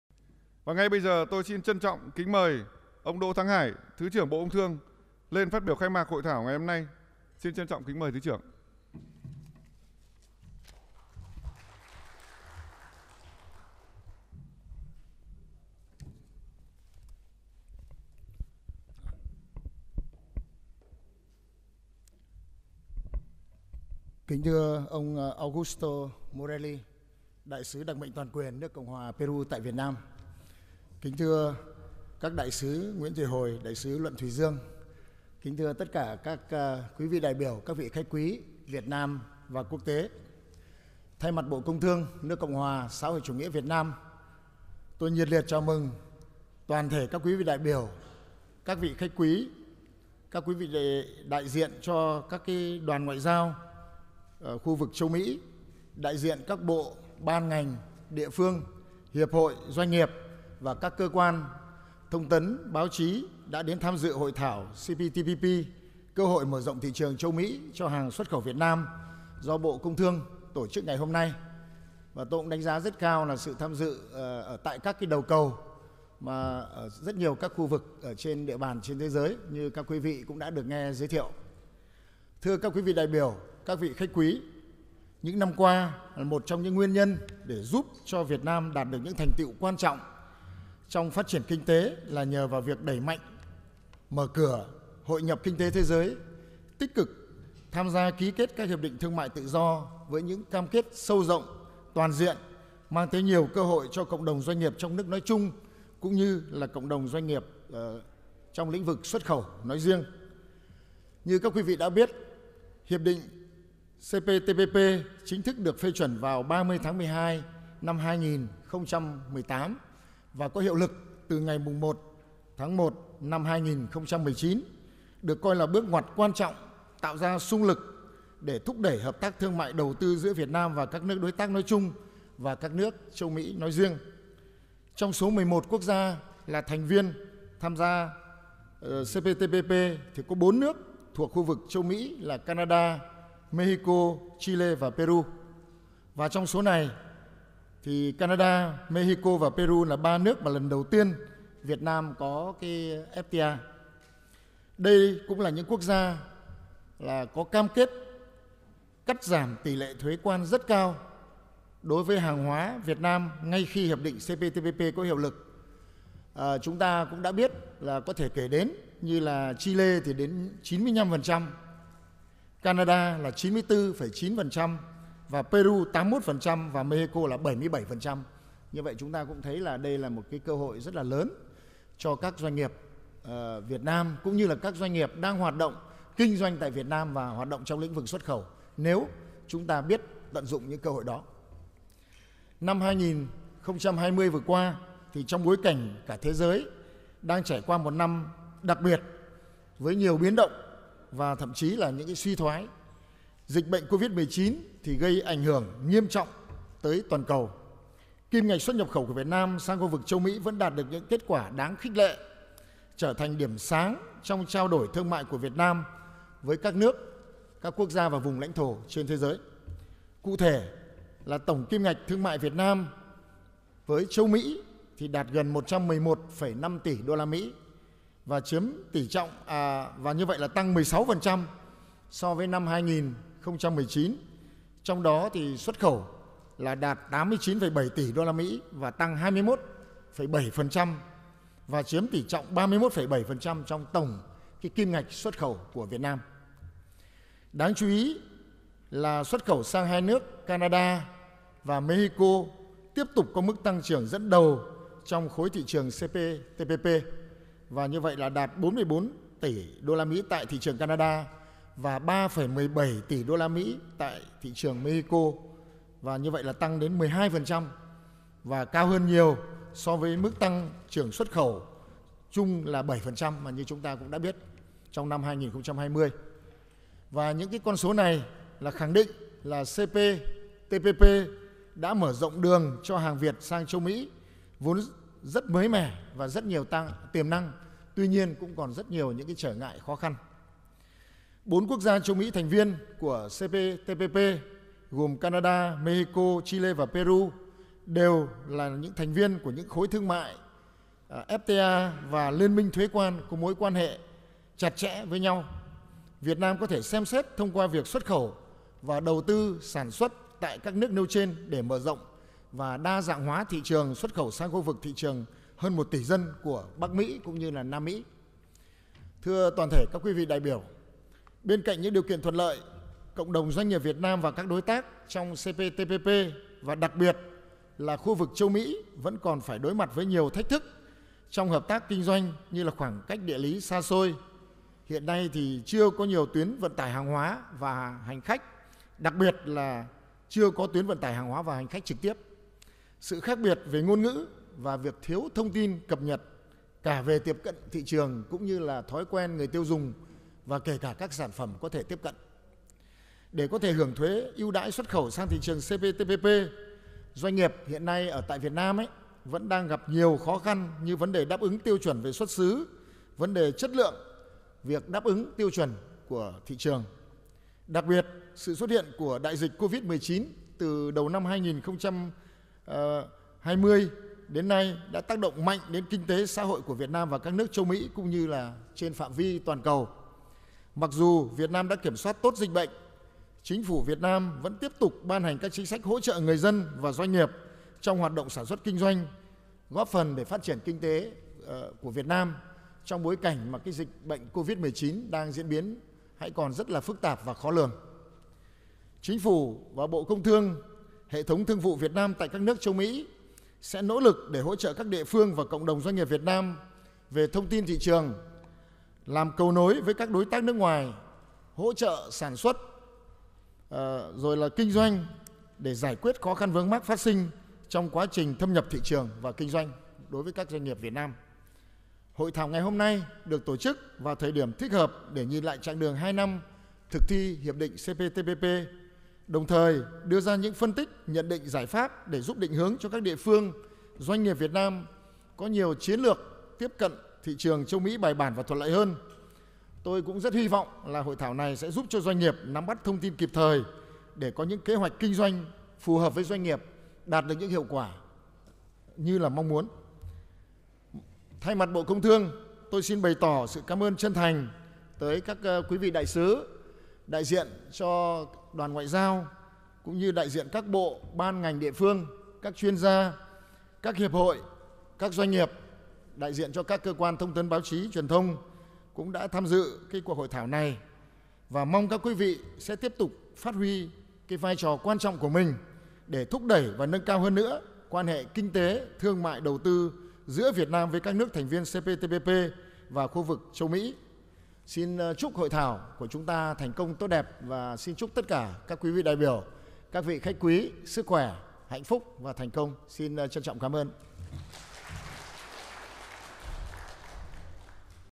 Phát biểu khai mạc của ông Đỗ Thắng Hải – Thứ trưởng Bộ Công Thương
Chương trình Hội thảo “CPTPP – Cơ hội mở rộng thị trường châu Mỹ cho hàng xuất khẩu Việt Nam”

Thời gian hội thảo: Ngày 27/4/2021
Đối tượng: Doanh nghiệp, hiệp hội và các cơ quan nghiên cứu, quản lý nhà nước